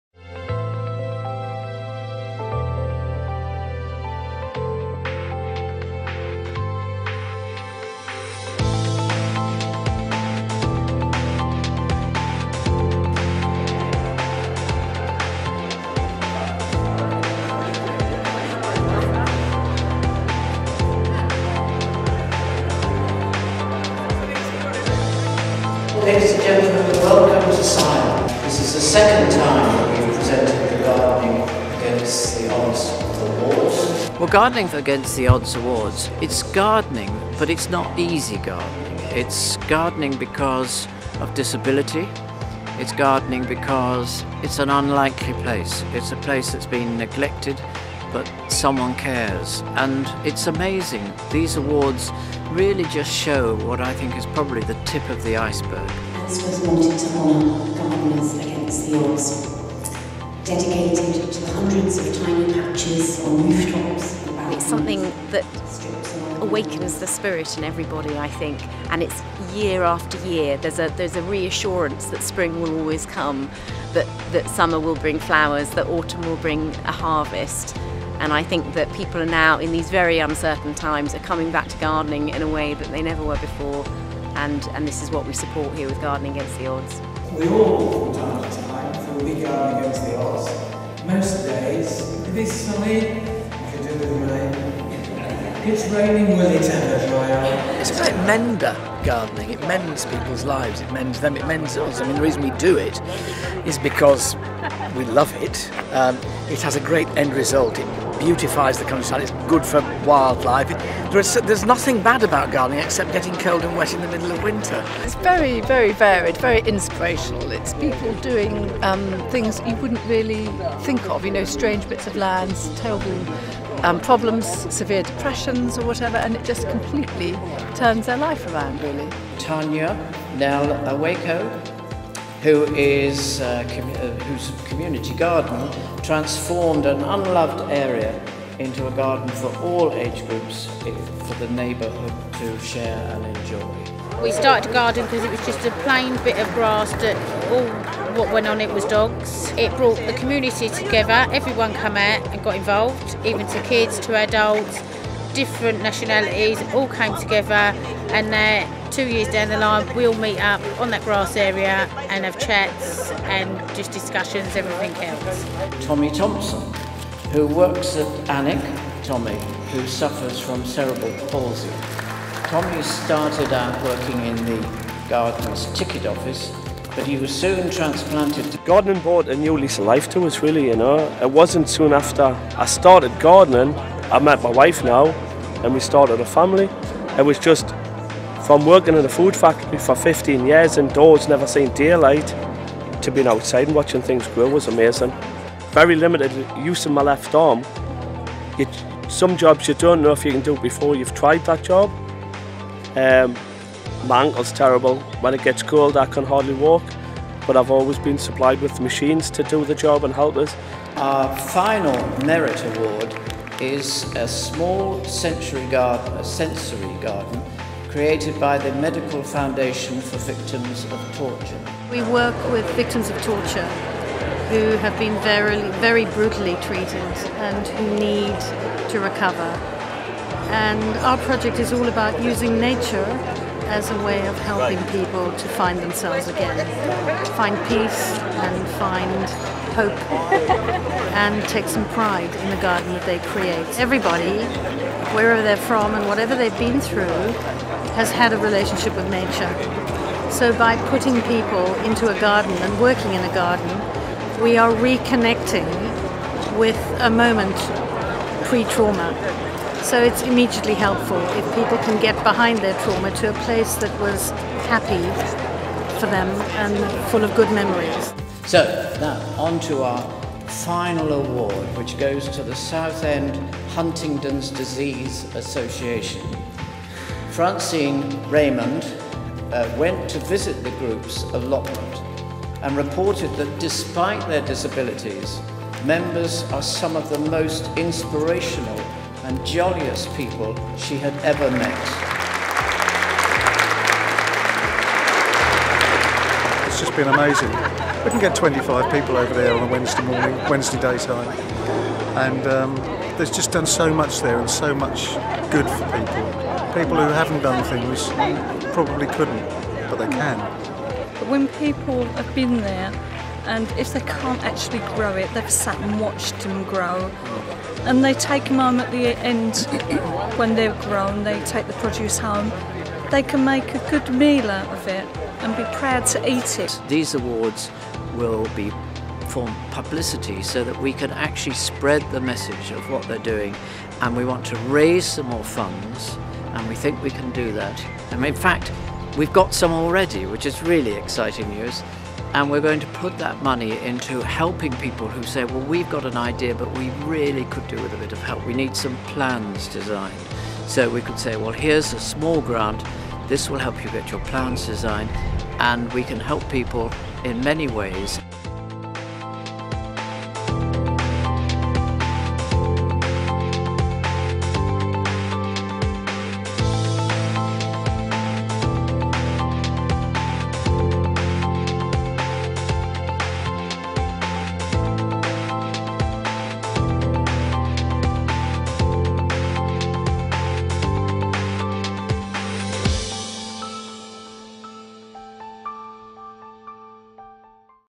Gardening Against the Odds - Awards Ceremony for 2011 Winners
Interviews and information on the March 2012 awards ceremony for the 2011 Gardening Against the Odds winners.